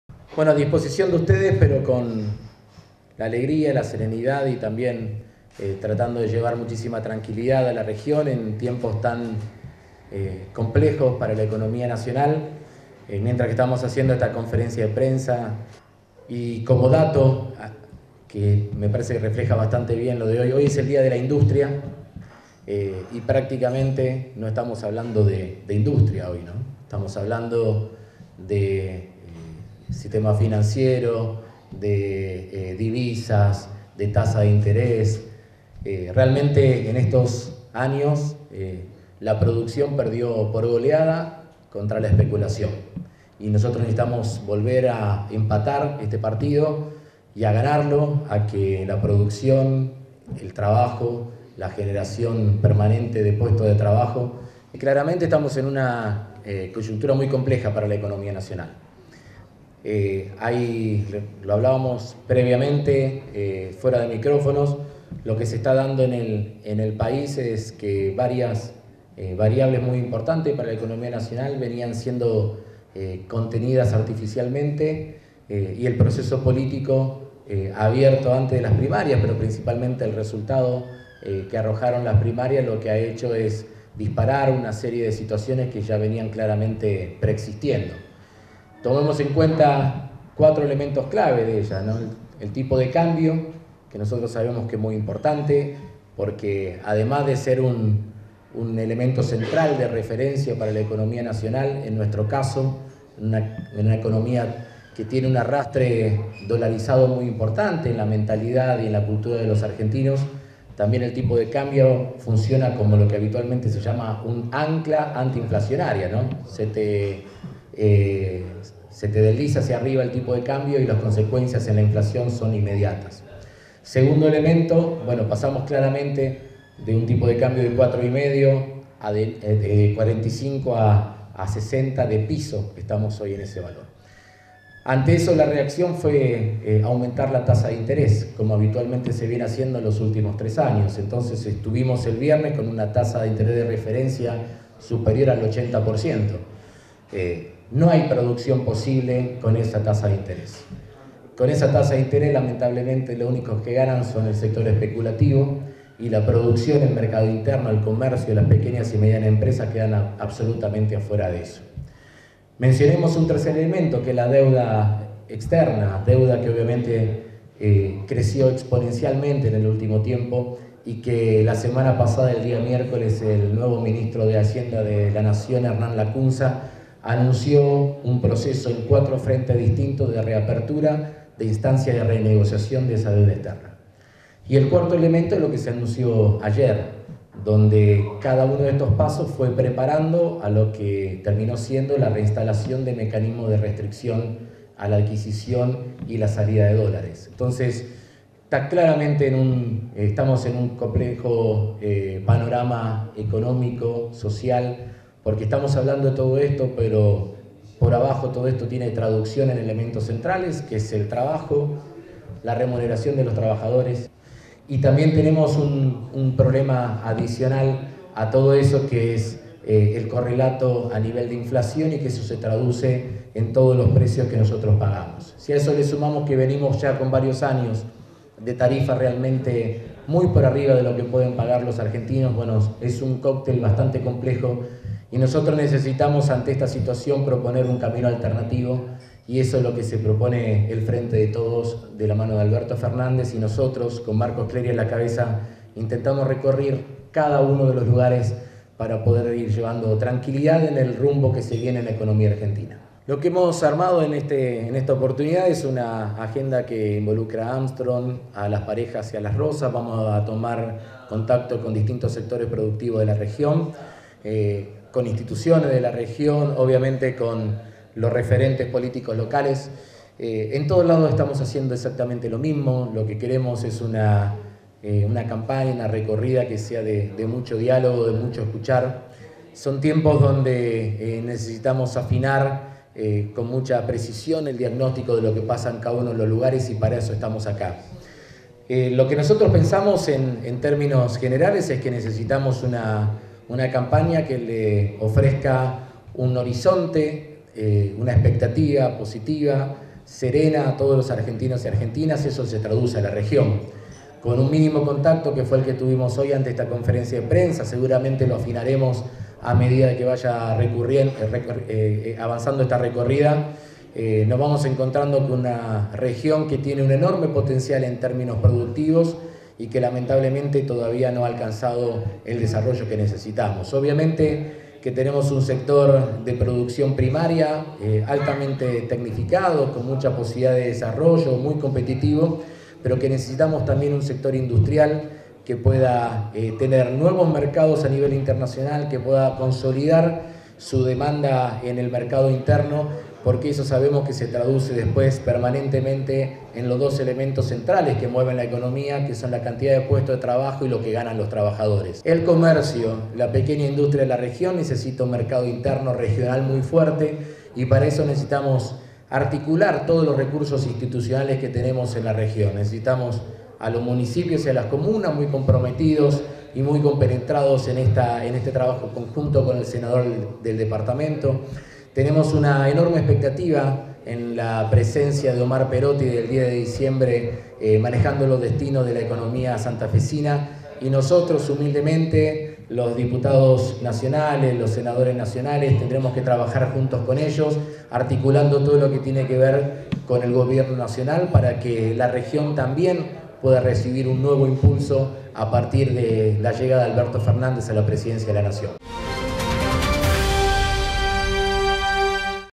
Política: el candidato Germán Martínez recorrió la región
El candidato a Diputado Nacional Germán Martínez, recorrió las industrias de Armstrong, Las Parejas y Las Rosas. En nuestra ciudad, el candidato por el Frente de Todos habló sobre las ultimas medidas económicas tomadas por el gobierno nacional.